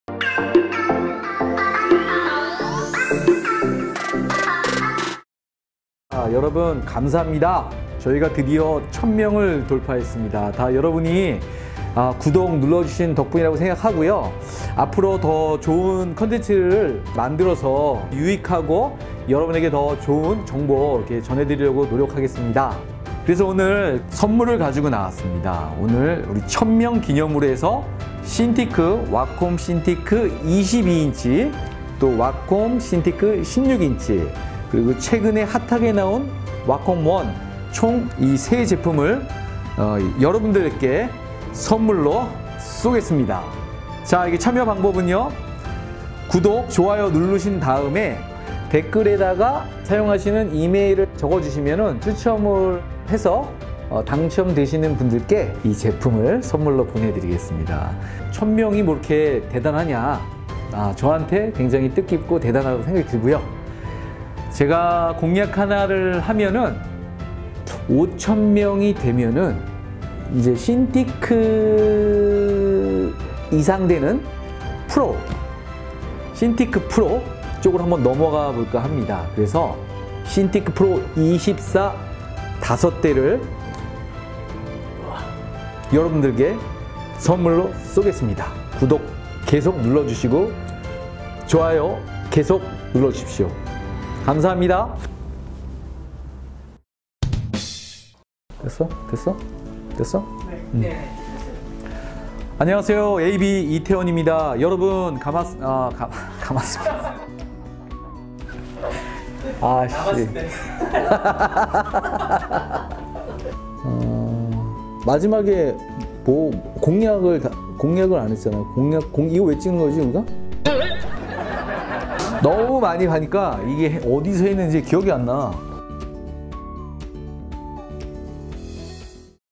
구독자 천명 달성 감사이벤트! '와콤 신티크 22' & '와콤 신티크 16' & '와콤 원' 애니벅스에서 액정타블렛 화끈하게 쏜다!!
구독자 1000 명 달성 이벤트로
애니벅스에서  대박 이벤트를
진행합니다.

무려 와콤 신티크 22, 와콤 신티크 16, 와콤 원을
이벤트 참가하신 분들 중 추첨을 통해 선물로 드립니다.

[이벤트 개요]
- 이벤트 기간 : 2020 년 2월 19일 ~ 3월 19일 (당일 16시까지)
STEP 1.  ab 애니벅스 유튜브 구독! (공개설정)
STEP 2. 1000명 달성 이벤트 영상에 좋아요!
STEP 3. 1000명 달성 이벤트 영상에 댓글!(이메일 필수)
(+ 이메일 @뒤에 '한글'로 작성해주시기 바랍니다)

- 사은품 : 와콤 신티크 22 / 와콤 신티크 16 / WACOM One  총 3대!

- 발표 : 3월 23일(월), ab 애니벅스 유튜브를 통해 공지
          * 이메일 별도 공지예정

- 사은품 배송 : 3월 25일(수) 택배 배송

*이메일 꼭 적어 주시고 / 구독자 공개 설정 꼭 해야합니다.